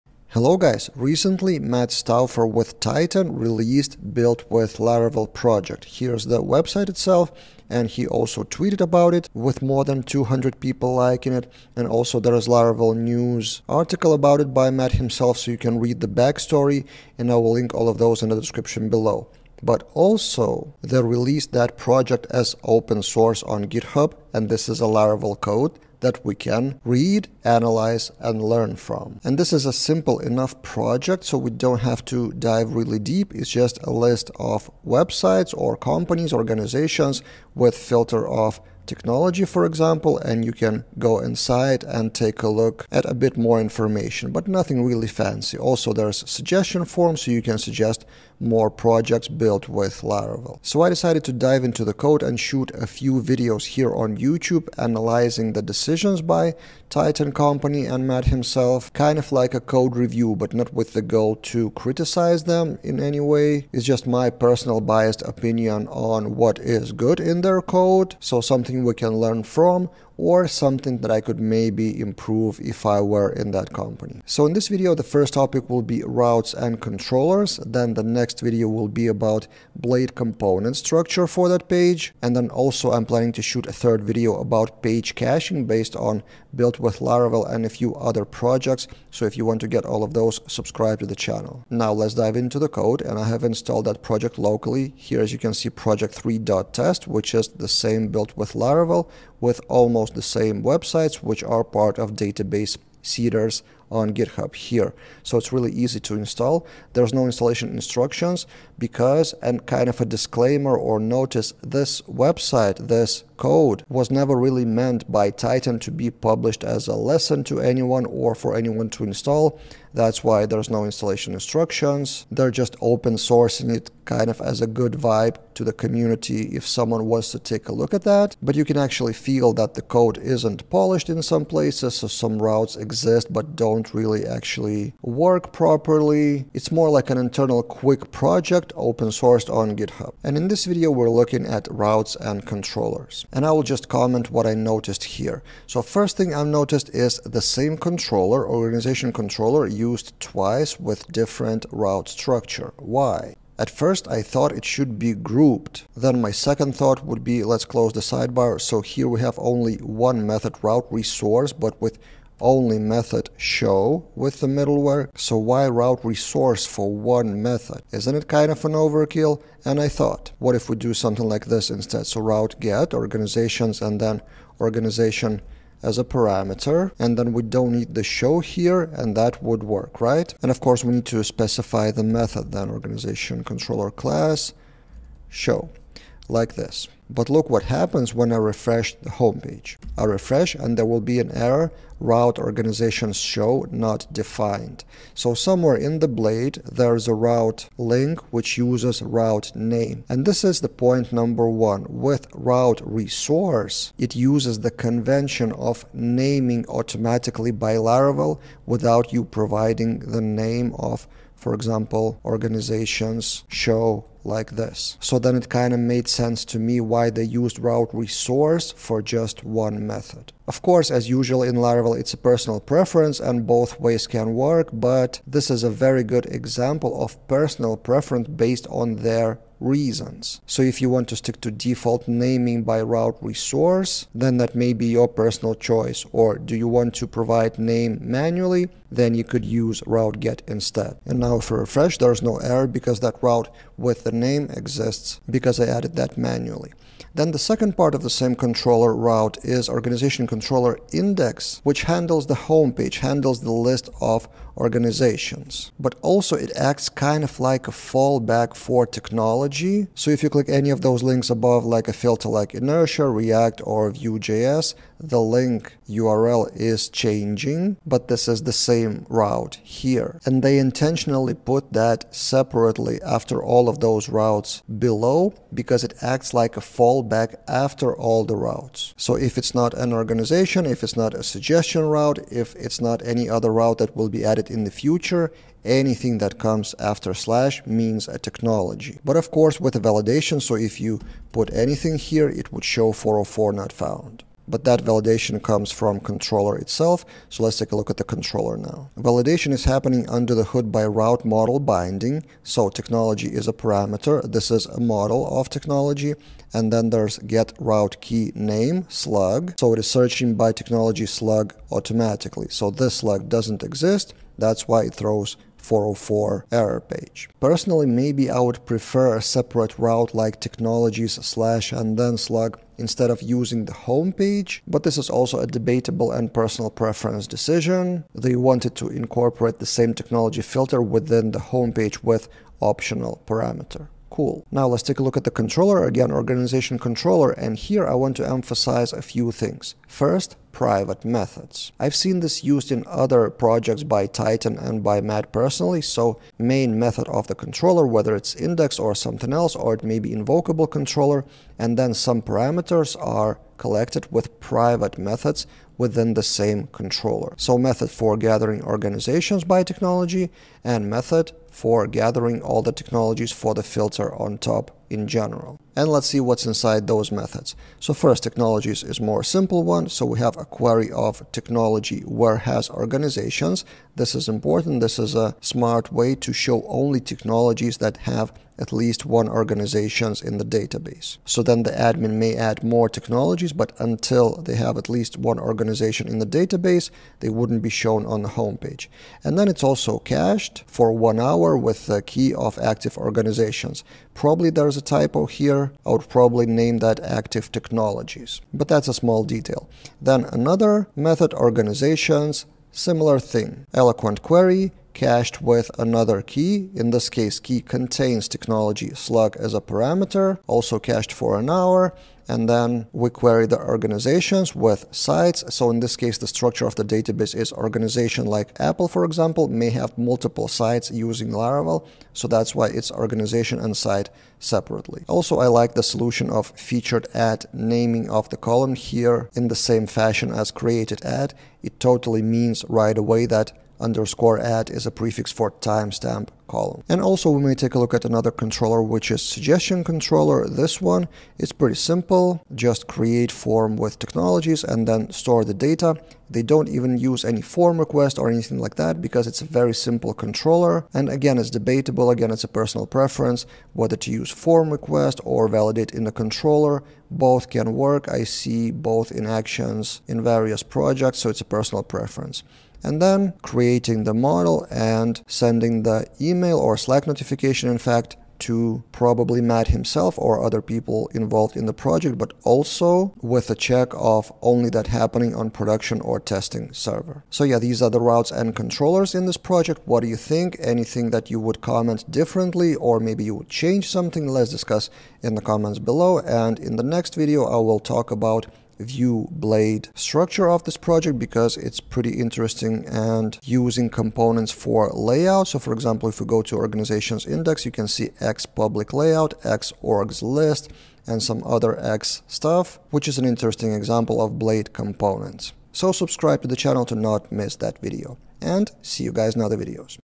"Built with Laravel" Code Review: Routes and Controllers
Start of a mini-series, reviewing the code of open-source project by Tighten called "Built with Laravel". In this video, we're talking about Routes and Controllers.